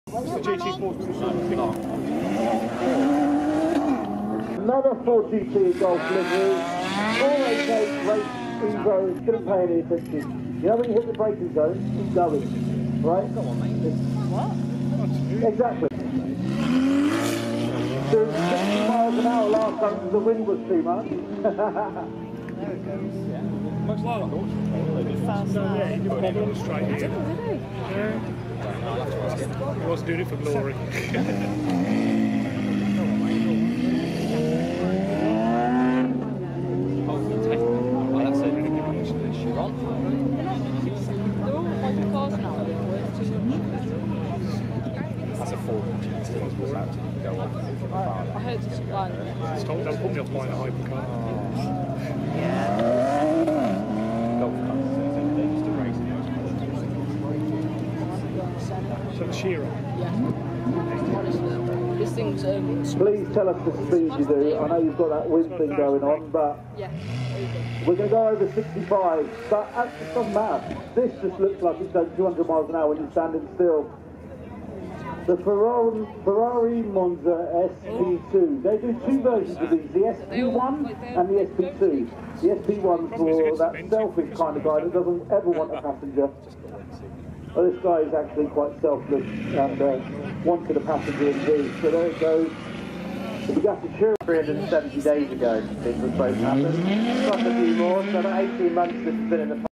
Hypercars launching at supercar fest (Koenigsegg Regera, Bugatti Chiron, Ferrari Monza and more)
Watch all the fastest and greatest hypercars launch at supercar fest.